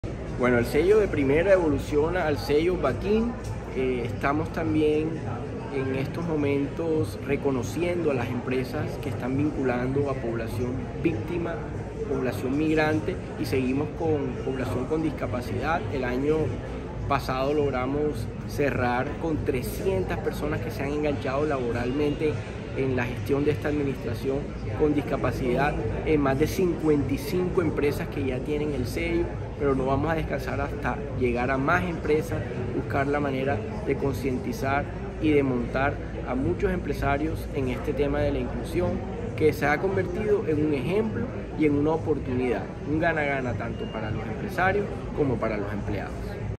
Sello BAQIn, la apuesta del Distrito por la inclusión laboral para población con discapacidad
Sello BAQIn, la apuesta del Distrito por la inclusión laboral para población con discapacidad, víctima y migrantes